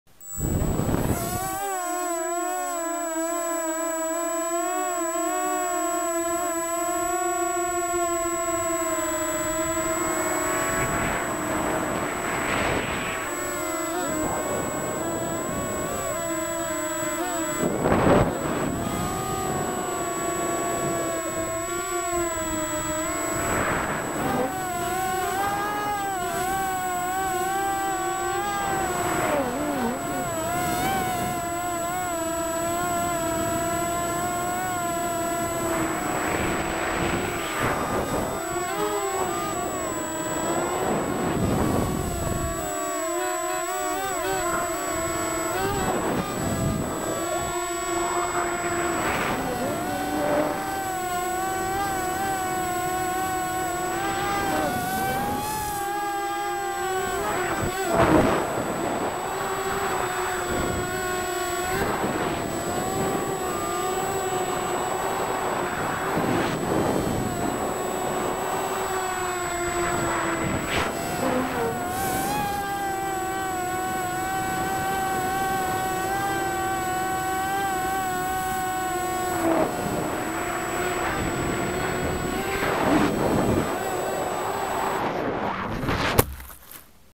Practice with a problem...
at the end it drops for no reason ...new battery checked it after it was at 3.94v